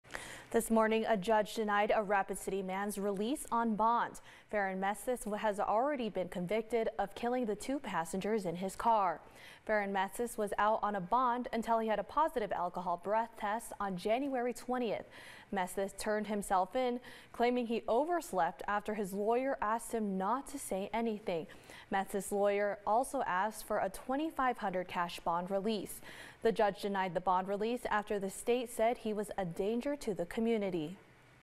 Rapid City man denied bond
Faron Mesteth was out on bond until he had a positive alcohol breath test on January 20.